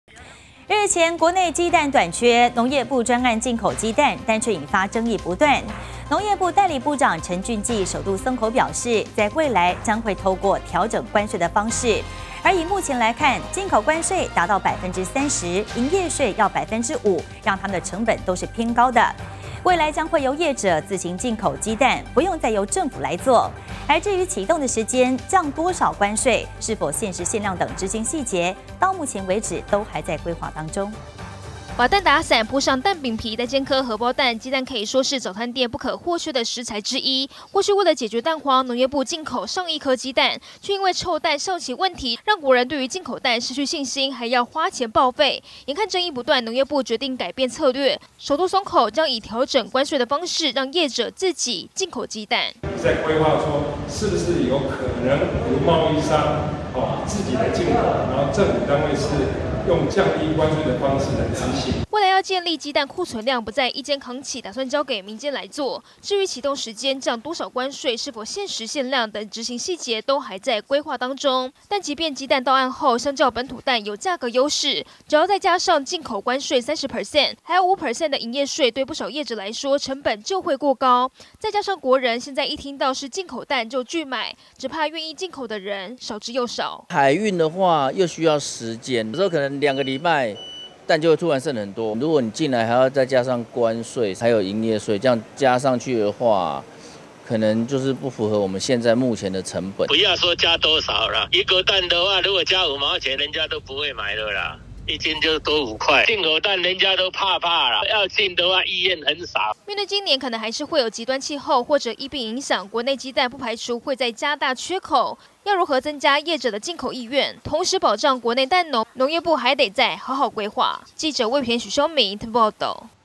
混充蛋掀波！農業部首降蛋關稅 進口還民間
日前，農業部專案進口雞蛋，引發一連串爭議。農業部代理部長《陳駿季》首度鬆口，未來將透過調整關稅的方式，由業者自行進口雞蛋。至於啟動的時間，降多少關稅，是否限時、限量等相關細節，目前都還在規畫中。